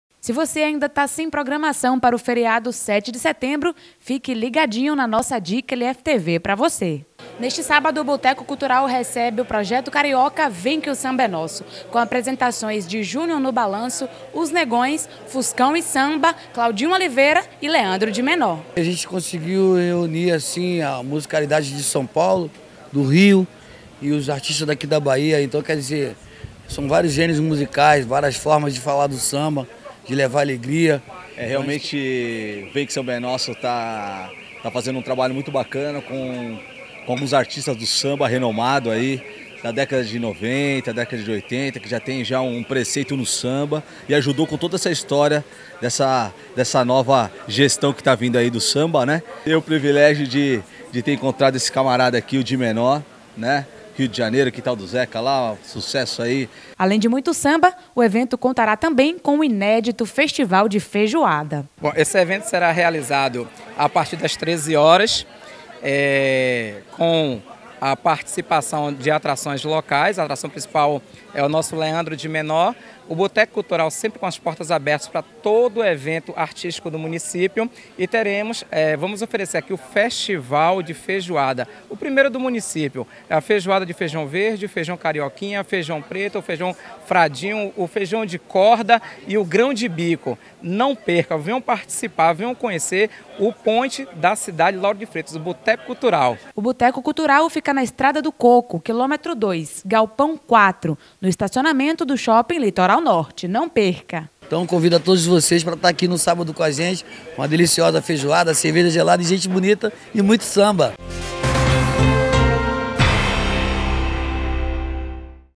Boteco Cultural recebe Vem que o Samba é Nosso
Neste sábado, feriado da independência, temos uma dica especial para você! No Boteco Cultural o projeto Vem que o Samba é Nosso agita a galera e ainda conta com uma variedade de feijoadas. Confira: